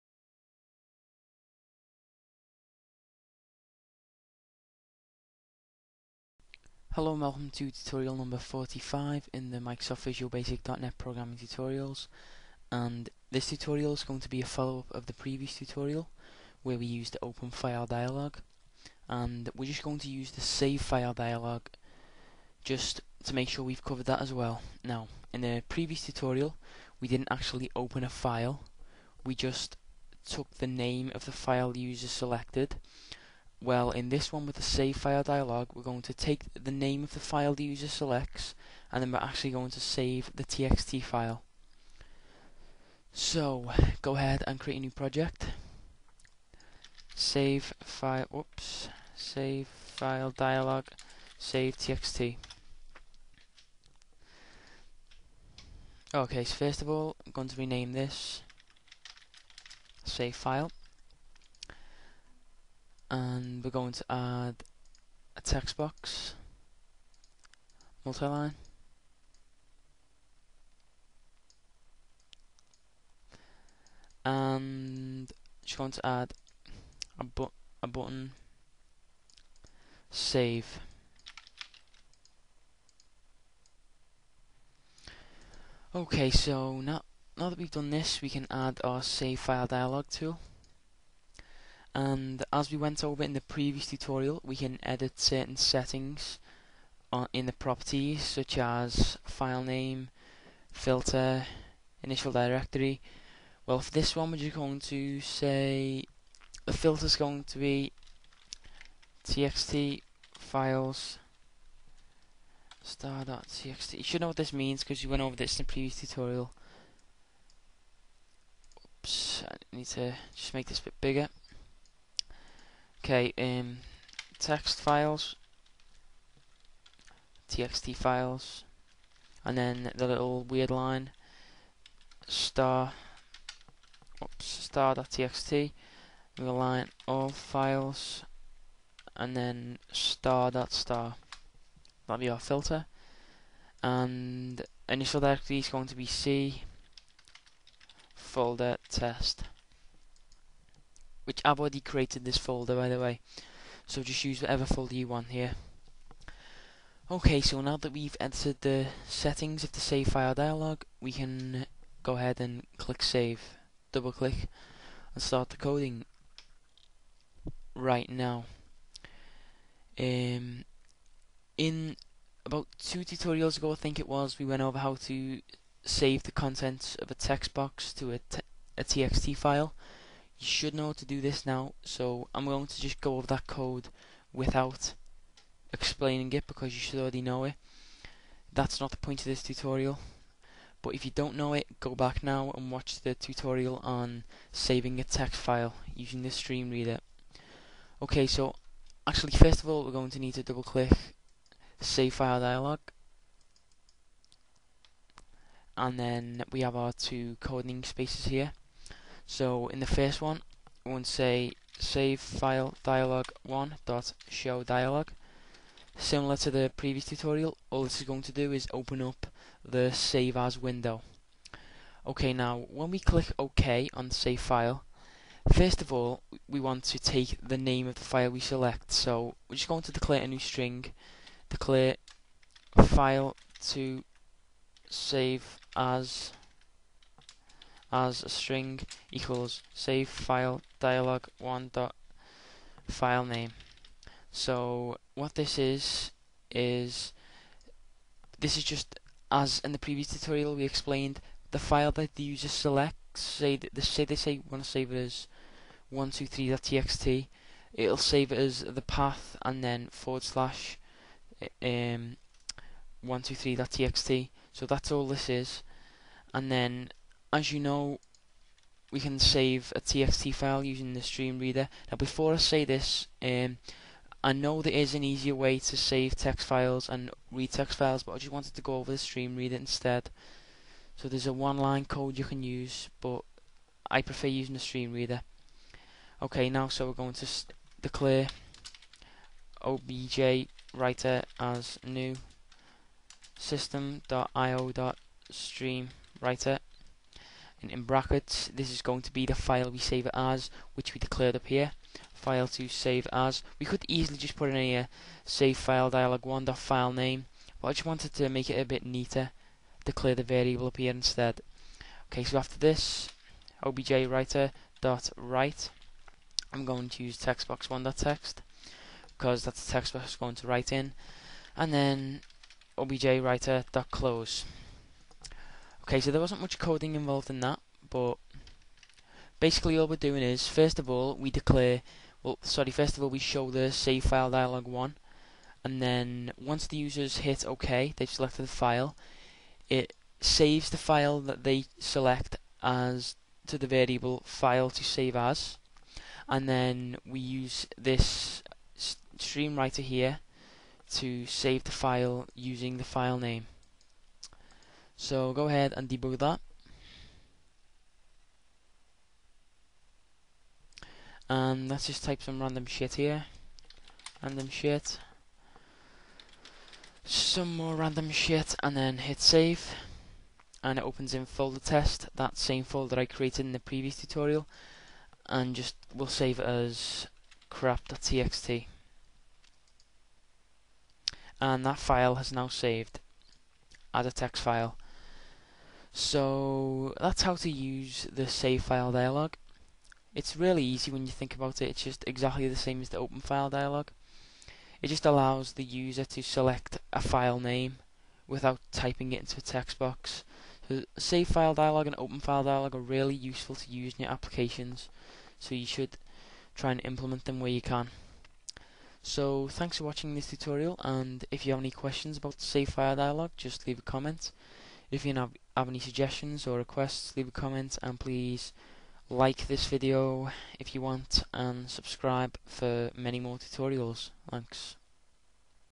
VB.NET Tutorial 45 - SaveFileDialog (Visual Basic 2008/2010)
This tutorial follows up the previous one and will teach you how to use the SaveFileDialog tool in Microsoft Visual Studio 2008. We use the savefiledialog to select a file name and then the streamwriter that we have previously covered to save a text file.